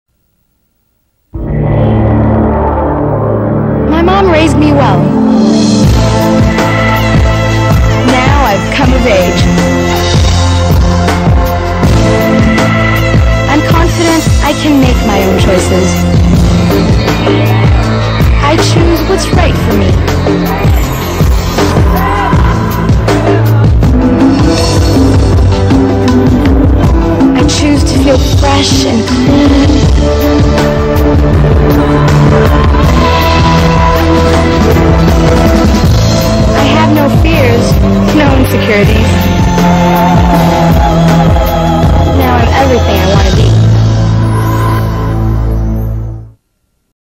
ph Care with KC Concepcion TVC 1
For everyday feminine care, pH Care
Featuring. KC Concepcion